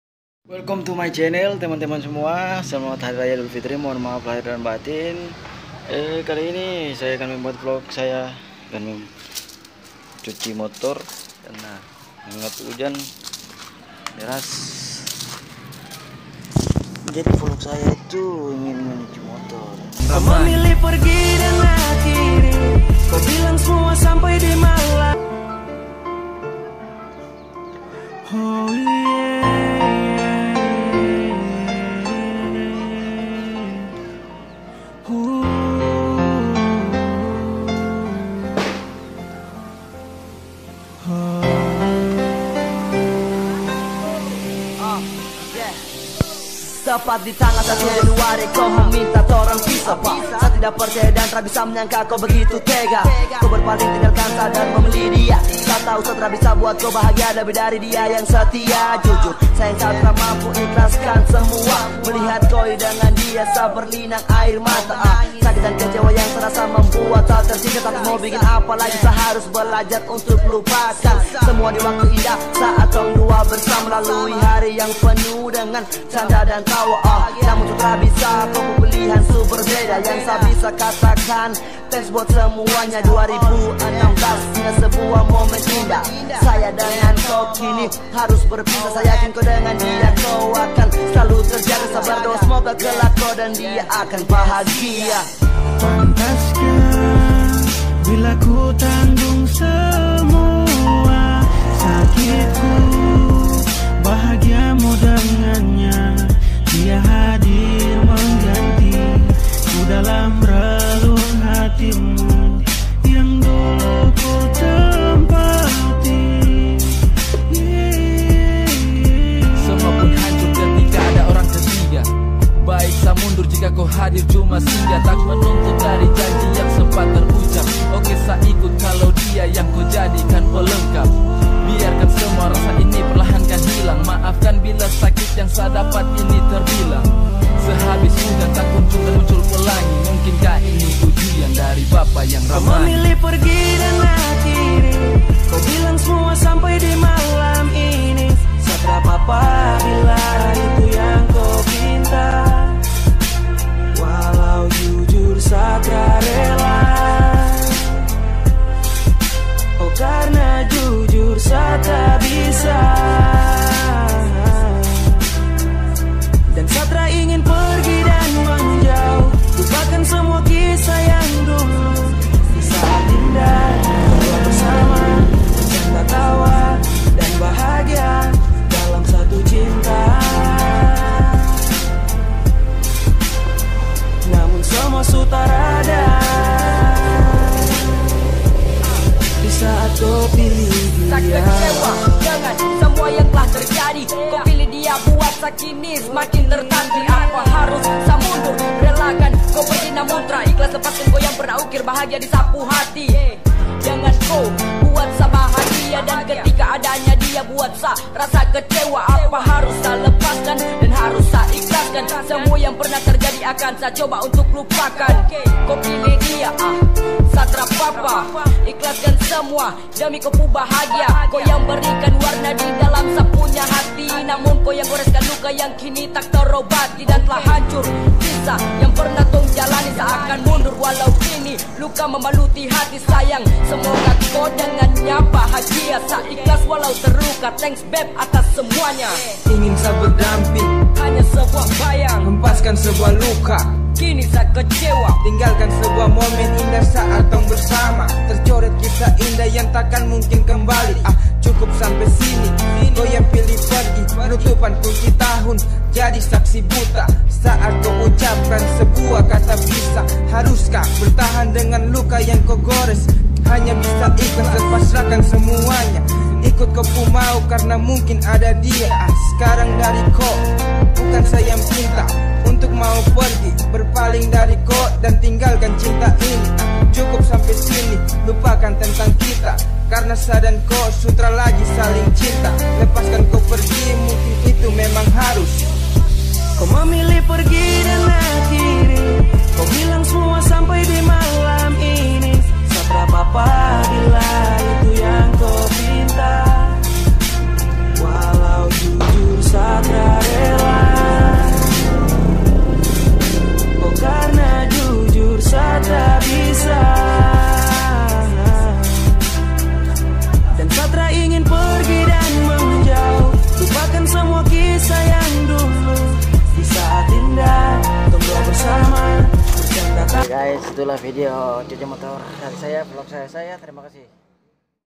NGEVLOG..!! CUCI MOTORNYA MANUAL
Didalam video ini saya ngevlog cuci motor secara manual..